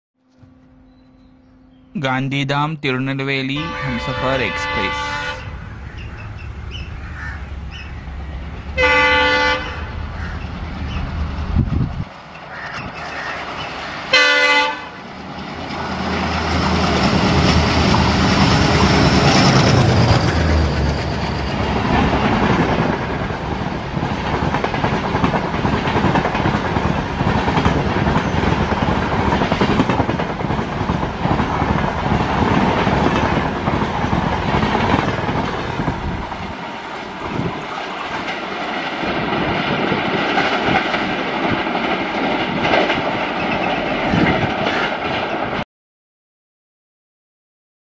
Terrific Konkan Railway : Raging Gandhidham Tirunelveli Humsafar Express with VTA ALCO Smashes Kudal
Konkan Railway / Indian Railways : 19424 / Gandhidham - Tirunelveli Humsafar Express hauled by Vatva (VTA) WDM3A honks and goes at terrific speed from Kudal (KR)

The 19423/19424 Tirunelveli - Gandhidham Humsafar Express is a superfast express train belonging to Western Railway zone that runs between Gandhidham Junction and Tirunelveli Junction in India.

It was announced to be operated with 19423/19424 train numbers on weekly basis.[2][3][4].

The inaugural special service of Gandhidham - Tirunelveli Humsafar Express (09424) was flagged off by Minister of States of Railways Rajen Gohain on 5 July 2018.

Location : Kudal (Konkan Railway), Sindhudurg, Maharashtra, INDIA

Keep Watching !!

Video by,
Prathamesh Yeram